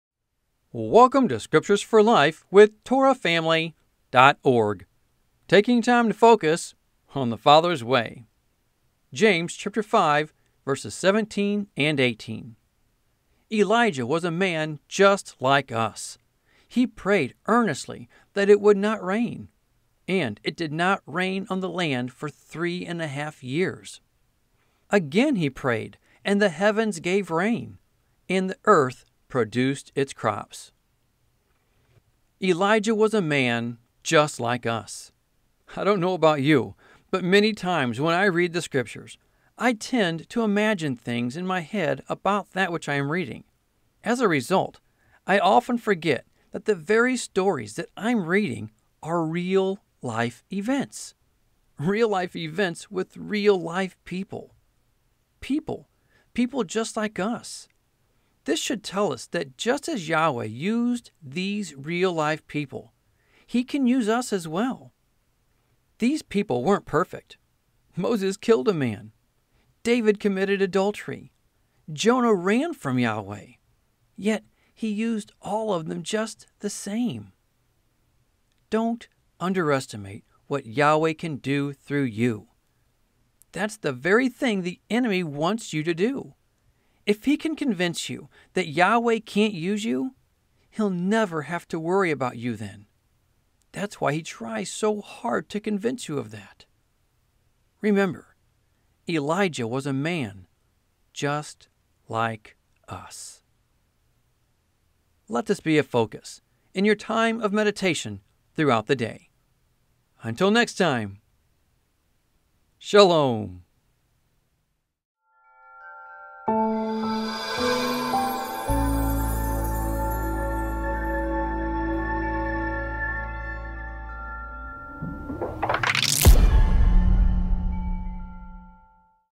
Scriptures for Life | Just Like Us
Daily devotion for June 8 (2 min)

James 5:17-18
Elijah was a man just like us. He prayed earnestly that it would not rain, and it did not rain on the land for three and a half years. Again he prayed, and the heavens gave rain, and the earth produced its crops.